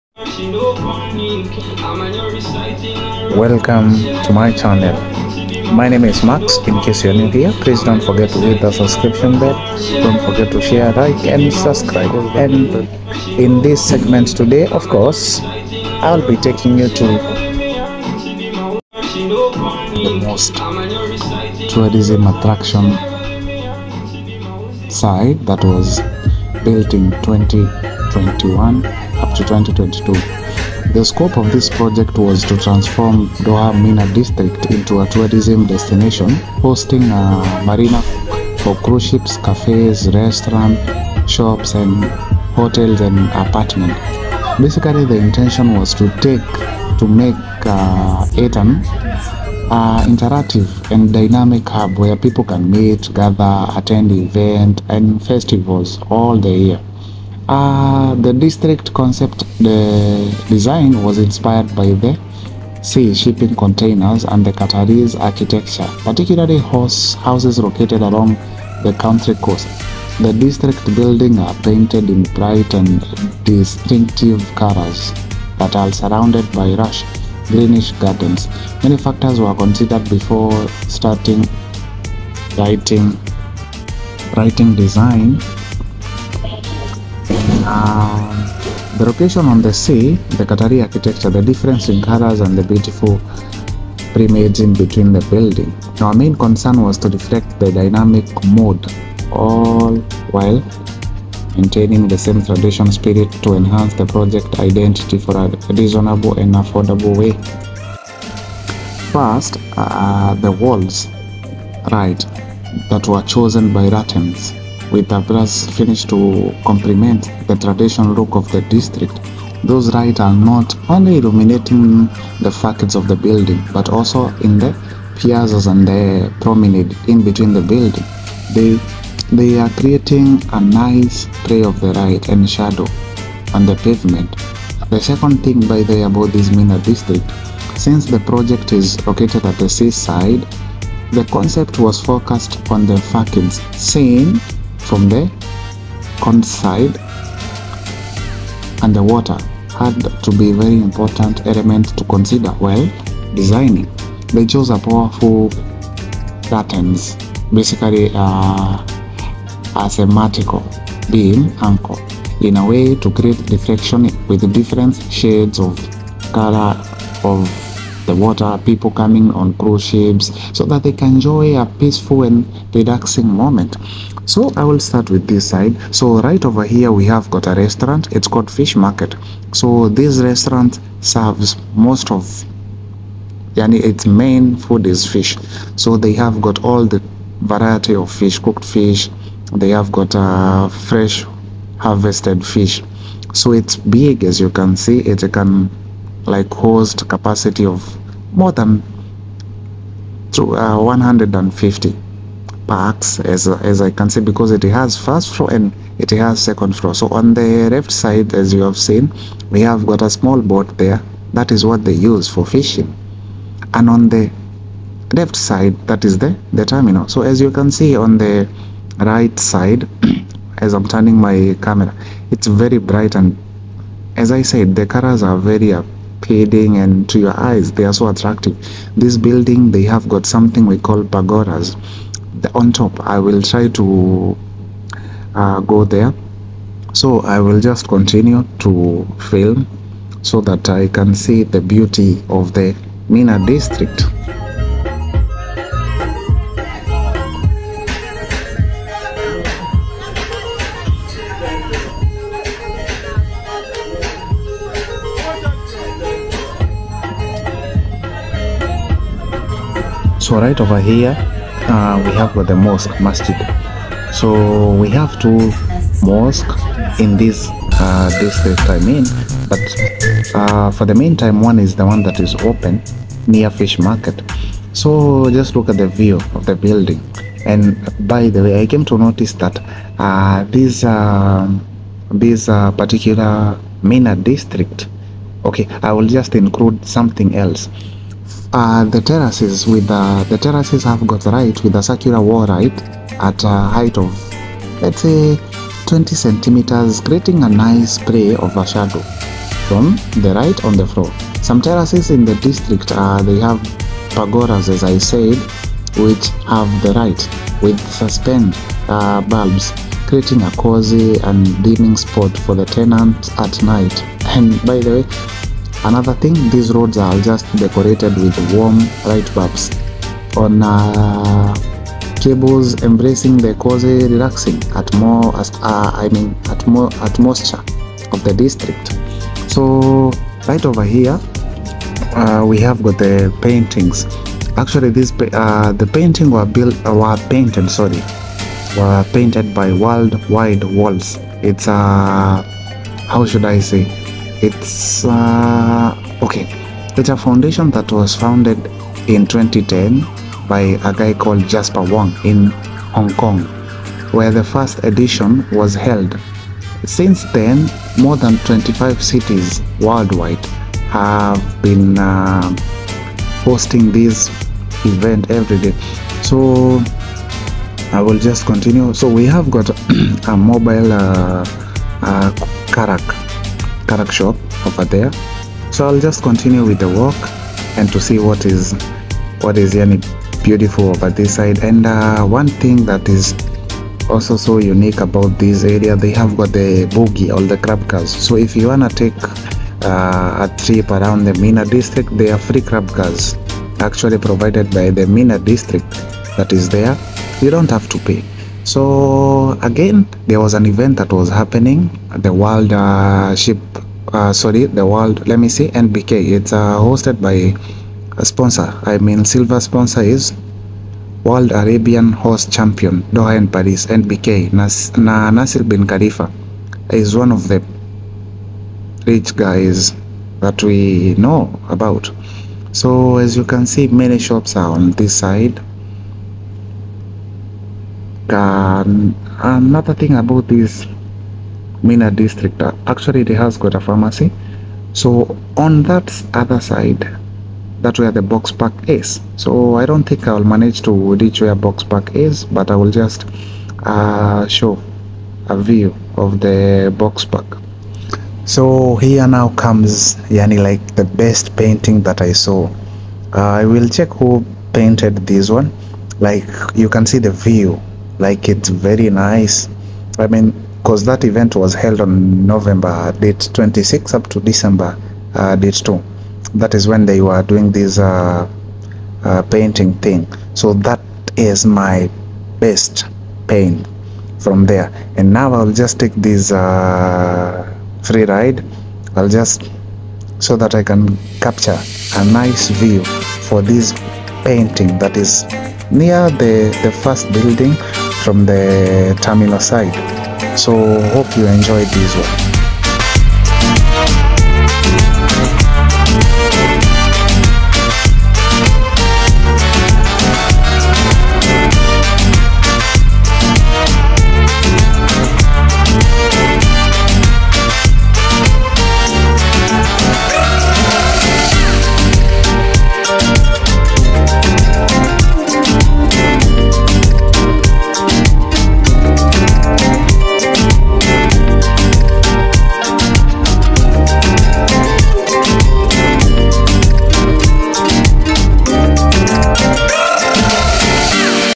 MINA🇧🇭|THE MAGNIFICENT MINA DISTRICT.Qatar's Little Santorini
Mina District, Qatar
IN 2022 – SPACES:HIGH BUDGET
Project Mina District - Old Doha Port
Location Doha, Qatar
Lighting Design MASQ, Qatar
Architect Erga Group, Qatar
Additional Design Dar Al-Handasah
Lighting Suppliers Fiberli, Egoluce, Lumisphere, Salvi Lighting, Ghidini

The scope of this project was to transform Doha Port Mina District into a touristic destination hosting a marina for cruise ships, cafes, restaurants, shops, and hotel apartments. The intention was to make it an interactive and dynamic hub where people can meet, gather, and attend events and festivities all year long.

The district’s concept design was inspired by the sea shipping containers and the Qatari architecture particularly houses located along the country’s coast. The district’s buildings are painted in bright and distinctive colors and are surrounded by lush green gardens.

Many factors were considered before starting our lighting design for this beautiful district; The location on the seaside; the Qatari architecture; the difference in colors and the beautiful promenades in between the buildings. Our main concern was to reflect the dynamic mood all while maintaining the same traditional spirit to enhance the project identity in a reasonable and affordable way.

Since the facades are painted with pastel colors already creating a dynamicity in the district, we decided to unify the color temperature (CCT-3000K) of all the fixtures.

• First, for wall lights, we chose antique Lanterns with brass finish to complement the traditional look of the district. Those Lights are not only illuminating the facades of the buildings but also the piazzas and the promenade in between the buildings. They are creating a nice play of light and shadow on the pavement.

• Second, since the project is located at the seaside, our concept was focused on the facades seen from the cornice side and the water had to be an important element to consider while designing. We chose powerful lanterns on the façade with asymmetrical beam angle in a way to create a reflection with different shades of colors on the water. People coming with the cruise ships will enjoy a peaceful relaxing mood.

• Third, all the stairs of the district are lit with small diameter circular step lights providing warm ambiance and enhancing the overall lighting in the space.

• Fourth, all the terraces are lit with circular wall lights at 20 cm height creating a nice play of shadow and light on the floor. Some terraces of the district have pergolas which we have lit with suspended light bulbs creating a cozy dining spot for tenants at night.

• Last, the roads are decorated with warm light bulbs on tensioned cables embracing